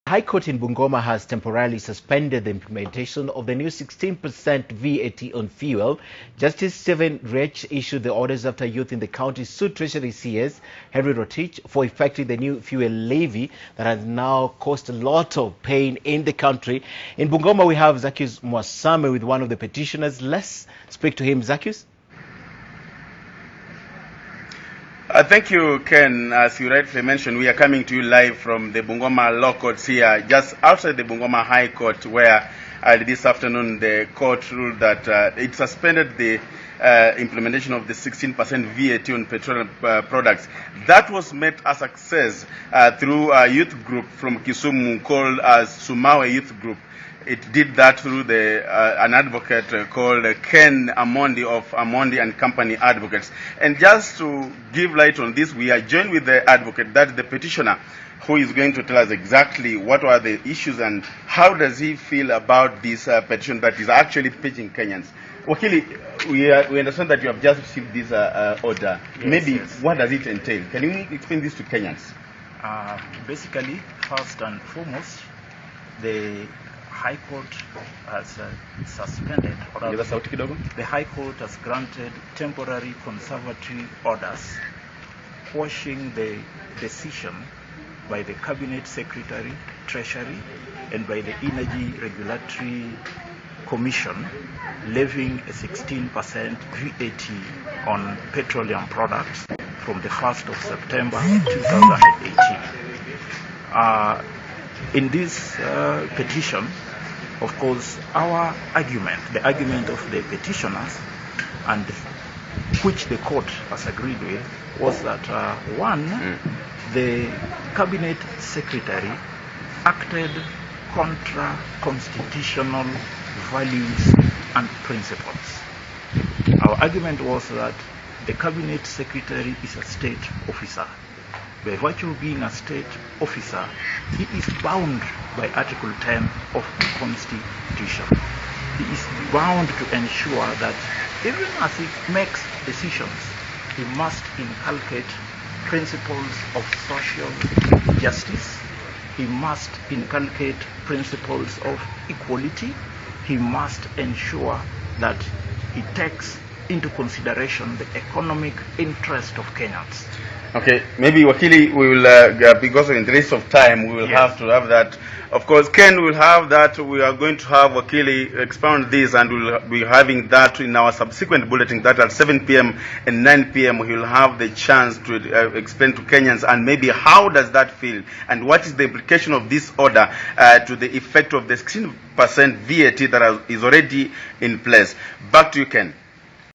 High Court stops new 16% VAT on fuel products
The High Court in Bungoma on Thursday granted temporary orders stopping the levying of 16% VAT on fuel products. The petitioners represented by Advocate Ken Amondi argued that the Treasury Cabinet Secretary Henry Rotich had flouted core constitutional principles and values in arriving at the decision which has caused a public outcry. Justice Stephen Riechi further ordered that the matter be mentioned on September 12, 2018 at the High Court in Kisumu.